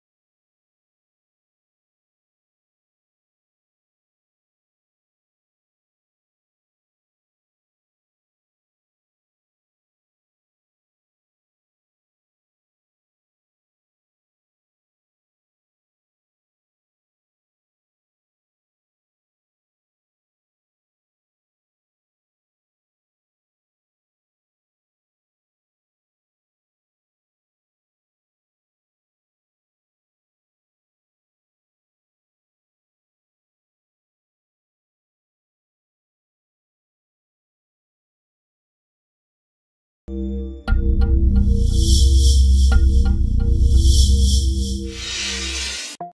THE MINAMI - 2023 World Cup in Doha (QAT) - MAG new FX
Performed successfully for value in the Finals of the Apparatus World Cup in Doha (QAT) Apparatus: Floor exercise by Kazuki MINAMI (JPN)
Element description: Double salto backward with 7/2 turn.
Element value: H value (EG III) # 66
Name awarded: The MINAMI

"Be sure to favorite and thumbs up the video!